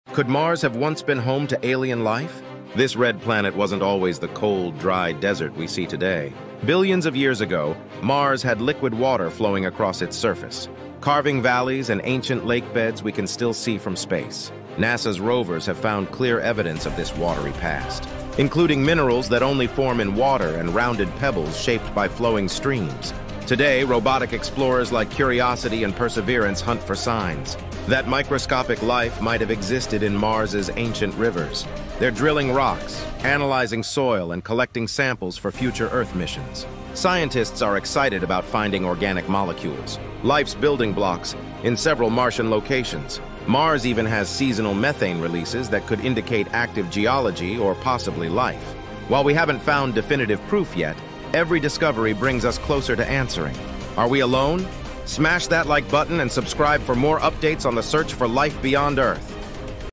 The Mars Paradox: What We Found and What It Means#Mars#Space#AlienLife#NASA#CuriosityRover#Science
This video explores the fascinating search for alien life on Mars, revealing how the planet once had liquid water and what recent rover missions like Curiosity and Perseverance have uncovered. We'll delve into the evidence of ancient rivers and the hunt for organic molecules, getting closer to answering the ultimate question: are we alone?